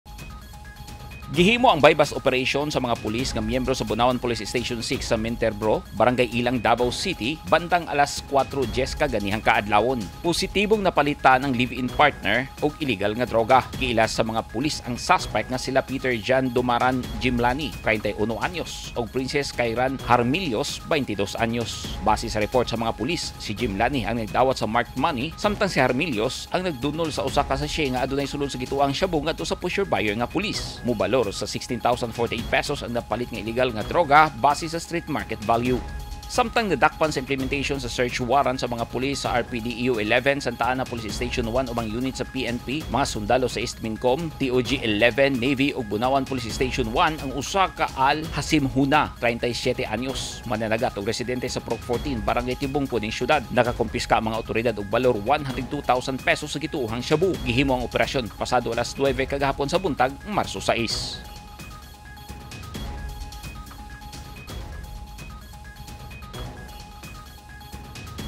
Live-in partner nga nagkasinabot nga magbaligya og gituohang shabu nasikop sa buy bust operation
Naaresto sa mga pulis ang live-in partner sa drug buy bust operation sa mga pulis sa Barangay Ilang, Davao City kaganihang kaadlawon, Marso 7.

Samtang gidakop sab sa mga otoridad ang usa ka mananagat human masakpan sa implementasyon sa search warrant nga nagtago og gituohang shabu sa Barangay Tibungco ning syudad.